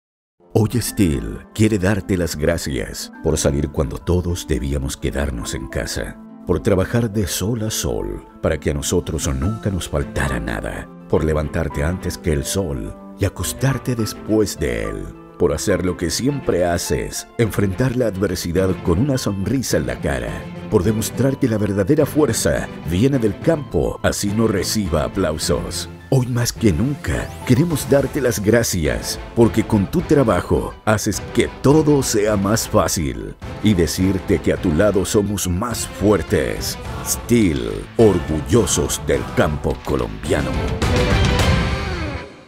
STIHL, orgullosos del campo colombiano
Cada vez estamos más convencidos de que la verdadera fuerza viene del campo, y agradecidos porque ante cualquier adversidad, siempre tendremos la sonrisa y pujanza de los que hoy siguen ConLaFuerzaParaSeguirAdelante.